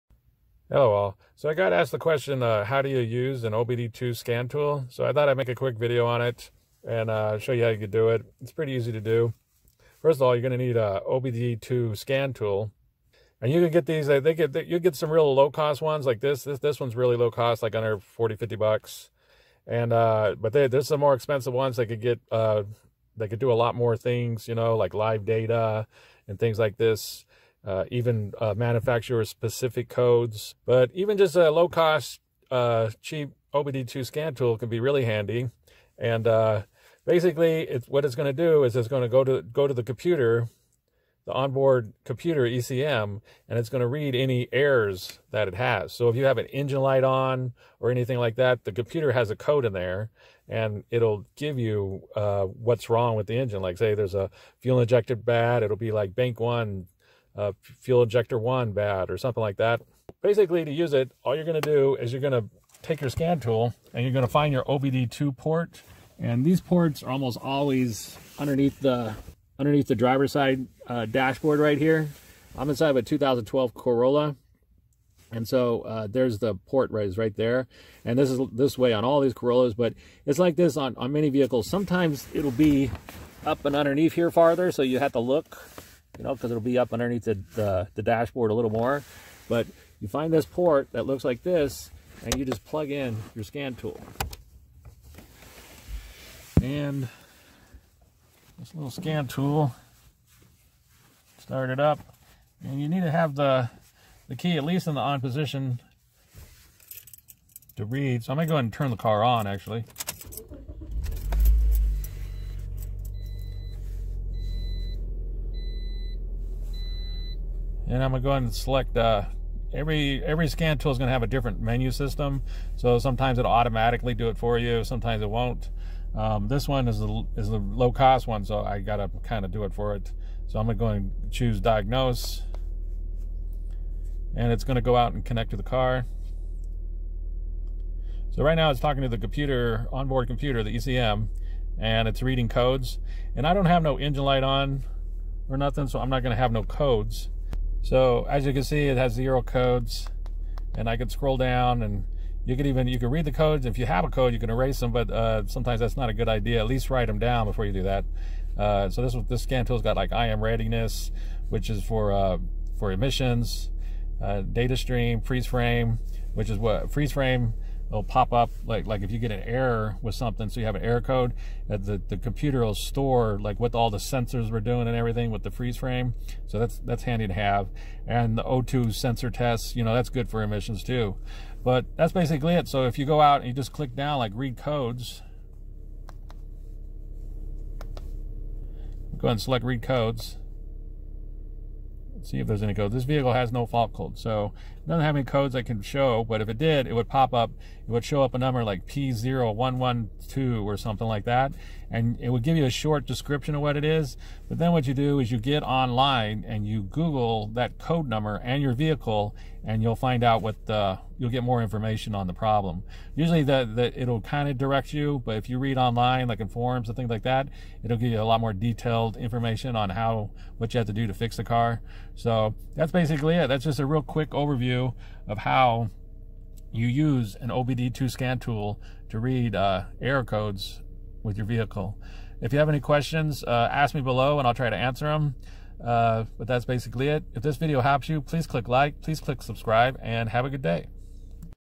How To Use an OBDII Scanner Tool to Read Engine Error Codes
Quick overview of how to use an OBDII scan tool to read engine dashboard light error codes.